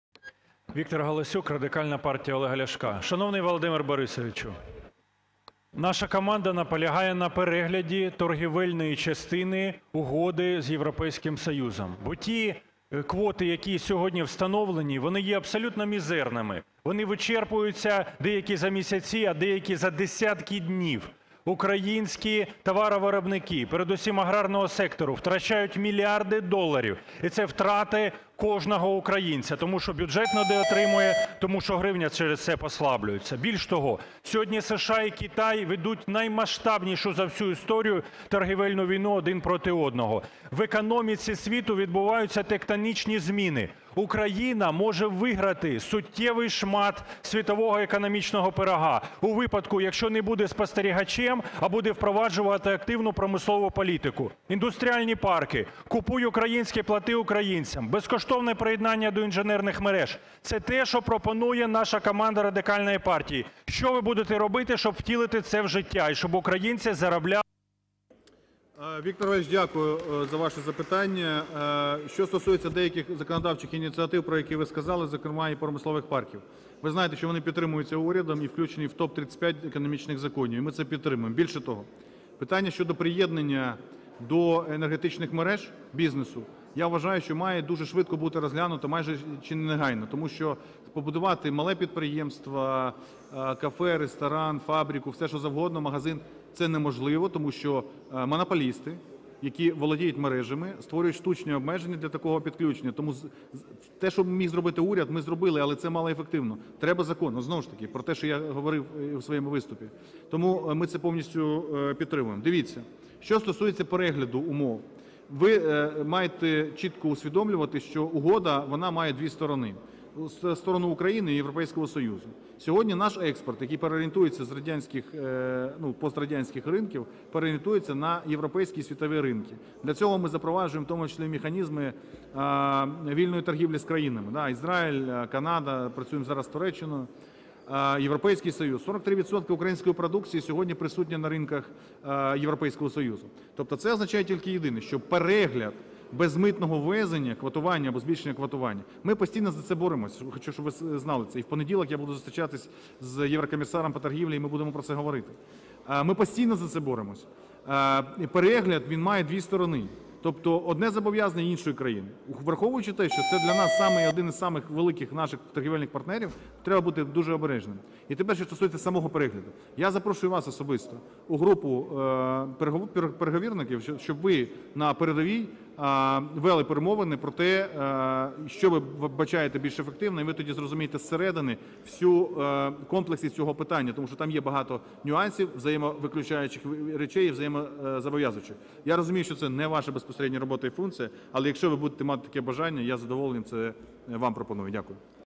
Галасюк-Гройсману: Давайте нарешті розвивати промисловість!
РПЛ вимагає перегляду торгівельної частини Угоди з ЄС. Про це під час Години запитань до Уряду заявив заступник Лідера РПЛ з економічної політики Віктор Галасюк.

Ті квоти, які сьогодні встановлені, є абсолютно мізерними й вичерпуються за місяці, ато й за десятки днів, пояснив голова Комітету з питань промислової політики та підриємництва.

"Українські товаровиробники, передусім, аграрного сектору, втрачають мільярди доларів. І це втрати кожного українця. Тому що Бюджет не доотримує, а гривня через це послаблюється", - зазначив В.Галасюк.

За його словами, сьогодні, коли США і Китай ведуть наймасштабнішу за всю історію війну один проти одного, в економіці світу відбуваються тектонічні зміни. Україна може виграти суттєвий шмат світового "економічного пирога" у випадку, якщо не буде спостерігачем, а буде впроваджувати активну промислову політику - індустріальні парки,  "Купуй українське, плати українцям!", безкоштовне приєднання до інженерних мереж".

"Це те, що пропонує наша команда РПЛ. Що Ви будете робити, аби втілити це в життя. Щоб українці заробляли більше?", - звернувся заступник Лідера РПЛ до Прем'єра.

На заклик В.Галасюка нарешті серйозно зайнятись промисловою політикою і підтримати ініціативи РПЛ Прем'єр-міністр відповів схвально, засвідчивши підтримку ПромПакетРеформ. Роботодавці, промисловці, підприємці чекають відповідних ДІЙ уряду.